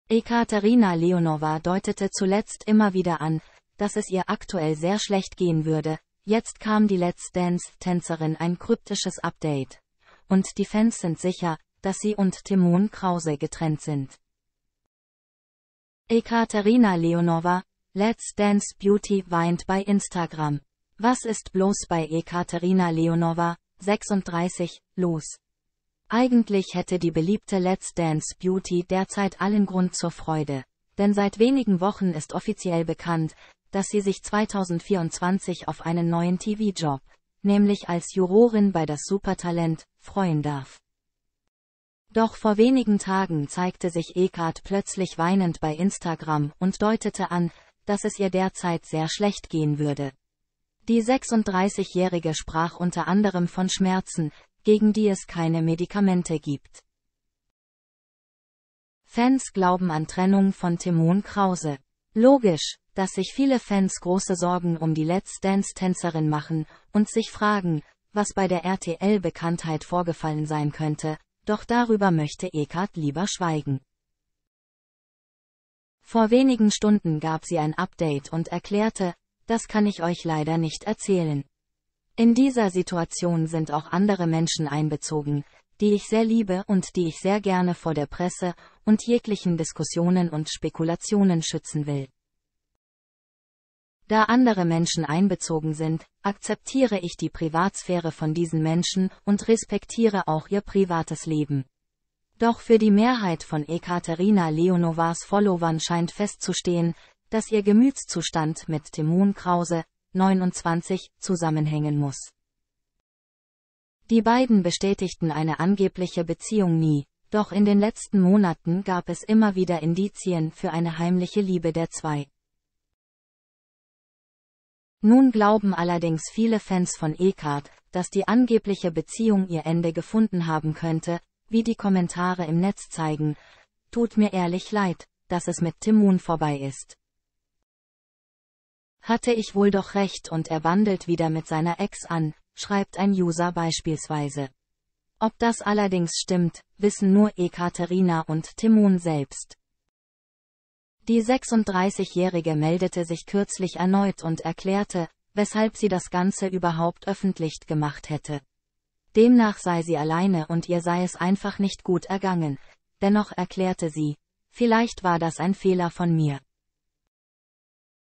Let's Dance Ekaterina Leonova Tut mir leid, dass es mit Timon vorbei ist
Ekaterina Leonova deutete zuletzt immer wieder an, dass es ihr aktuell sehr schlecht gehen würde, jetzt kam die "Let's Dance"-Tänzerin ein kryptisches Update - und die Fans sind sicher, dass sie und Timon Krause getrennt sind.